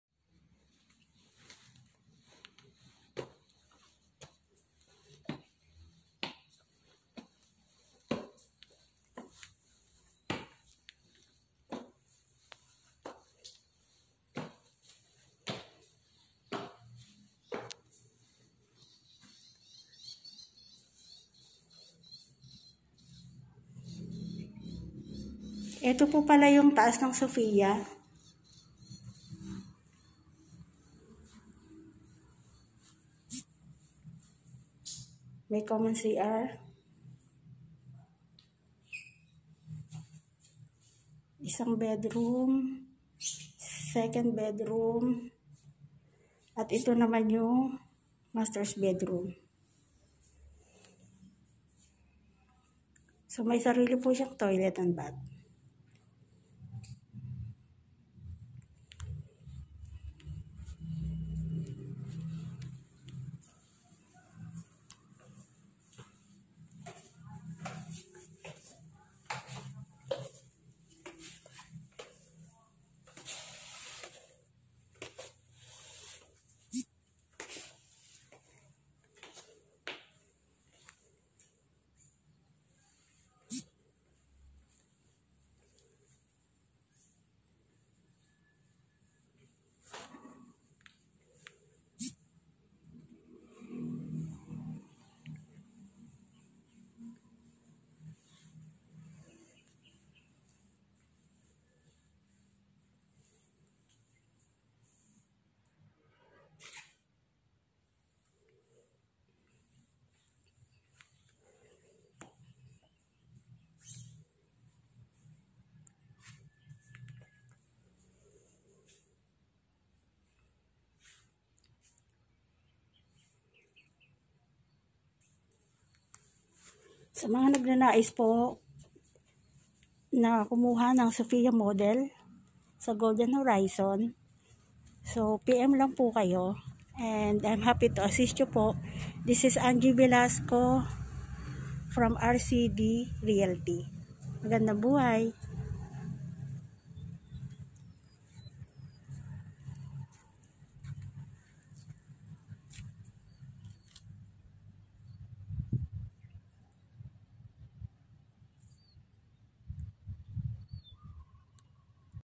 SOFIA MODEL IN HUGO PEREZ TRECE CAVITE. FOR INQUIRIES
🧡2Storey Townhouse with carport.
🧡near SM Trece,school,market,
🧡hospital and more commercial establishent
Happy to assist  you.😊